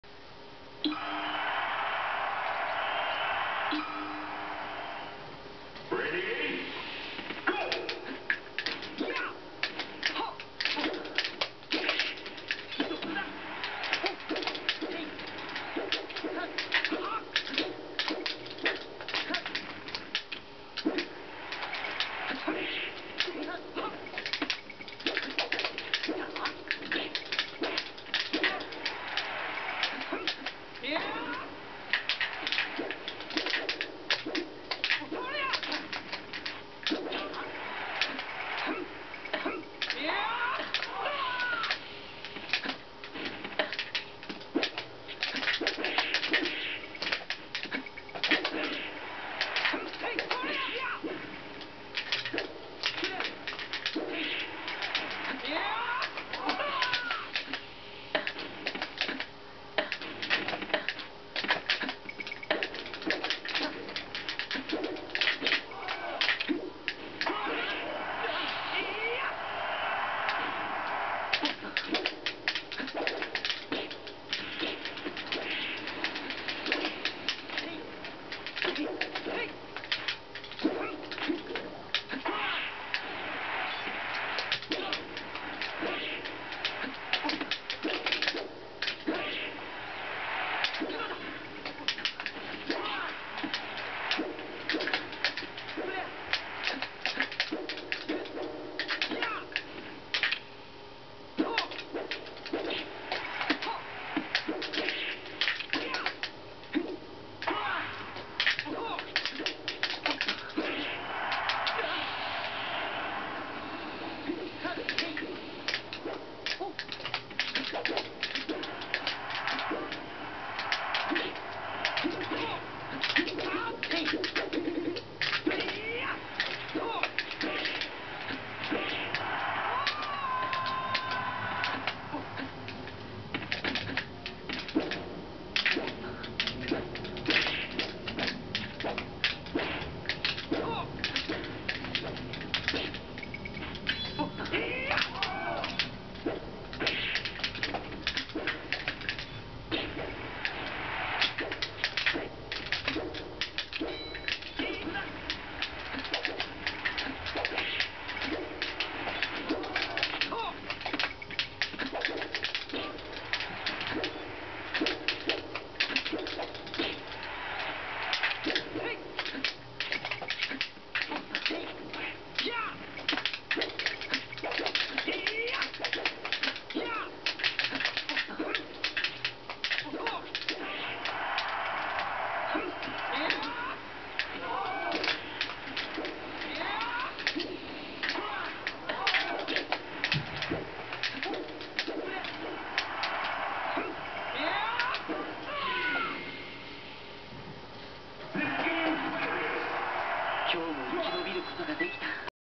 MooseEatsBear(Marth) vs. DemonEye(Roy)
Match recorded today. Attempt to combo for video.